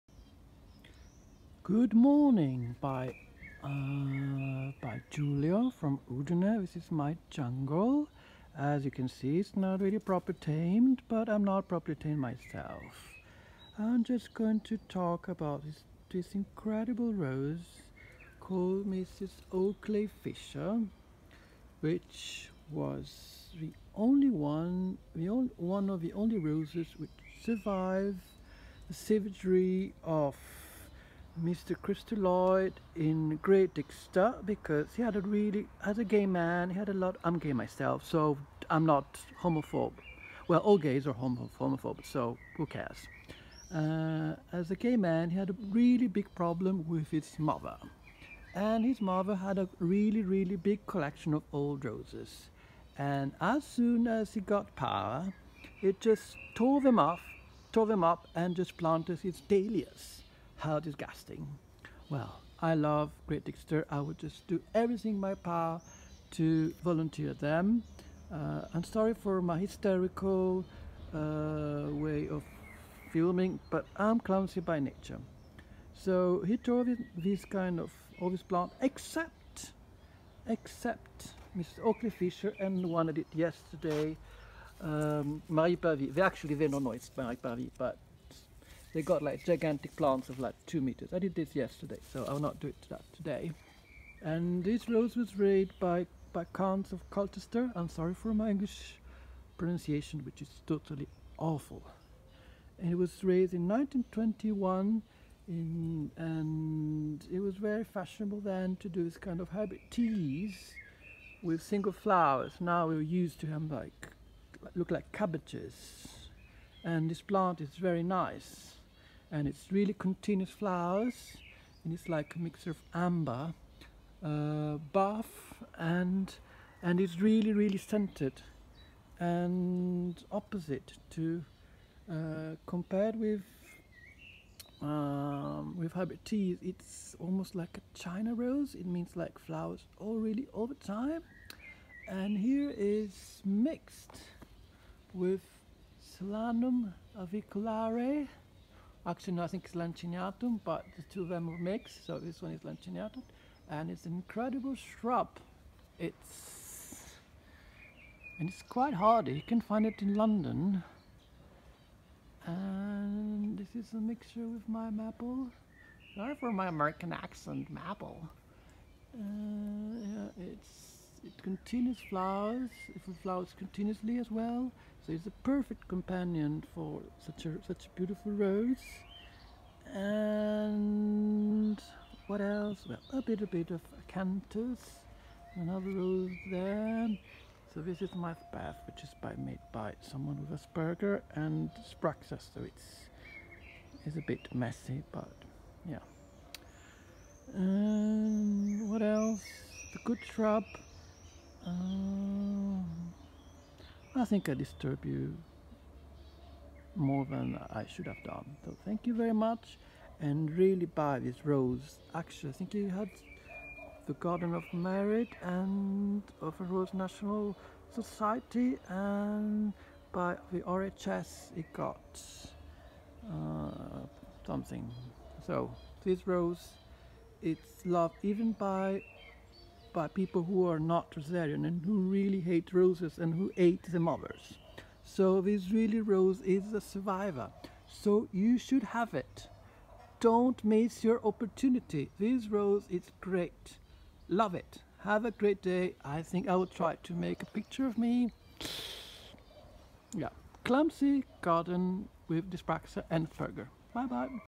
the rose which survived Cristopher Lloyd’s savagery and barbarity
Mrs Oakley fisher a rose which survived Christopher’s Lloyd s barbaric savagery